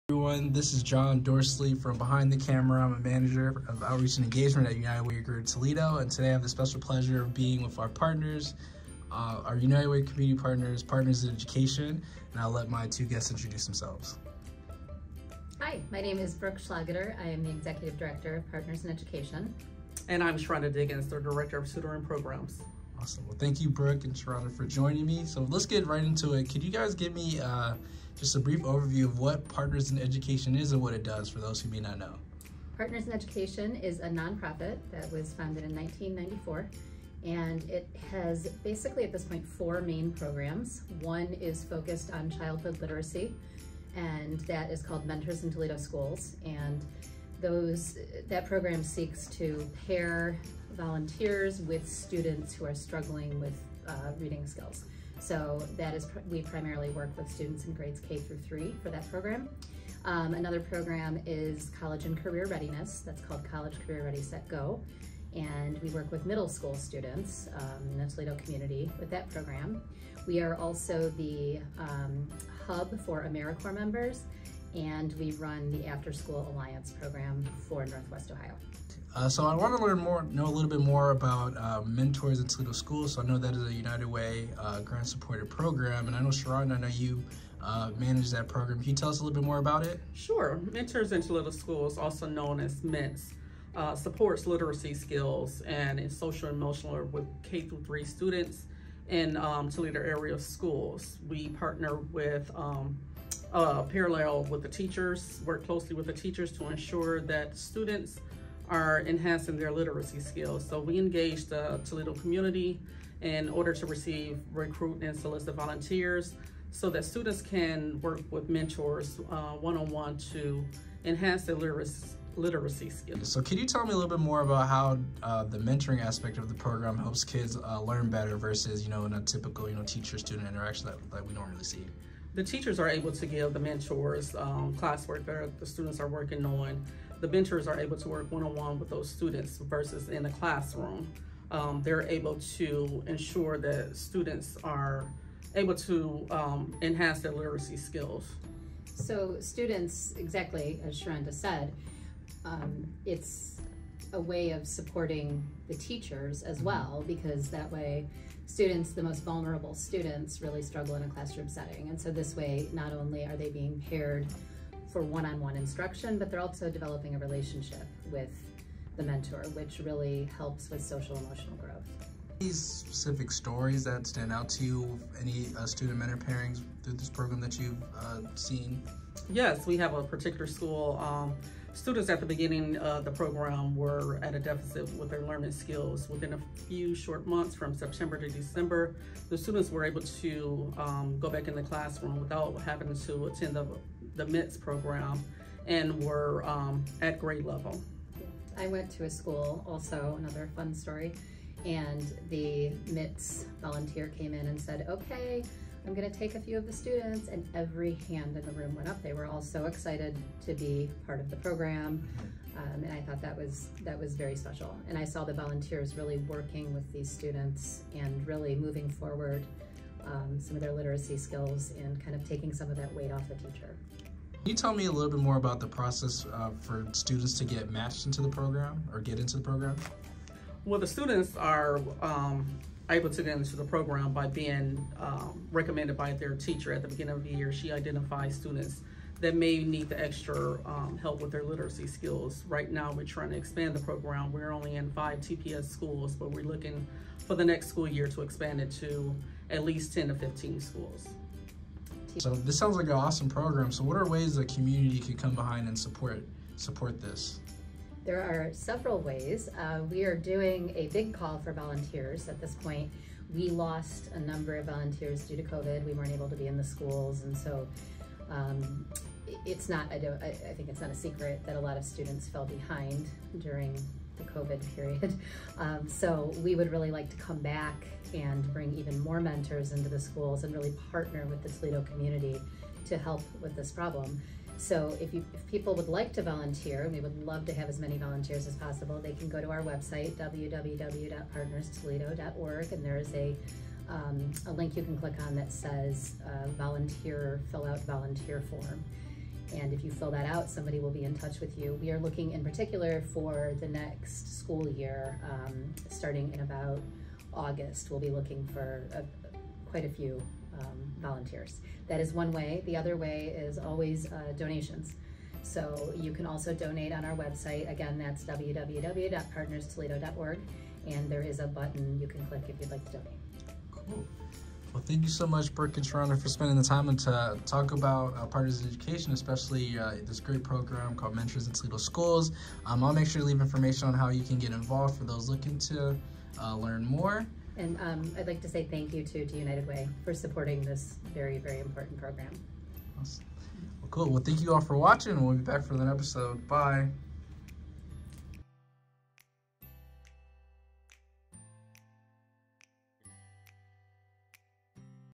The Uniters - Partners in Education
Brooke Schlageter and Sharonda Diggins from Partners in Education, met with Jon Dorcely to talk about their organization and specifically their program, Mentors in Toledo Schools.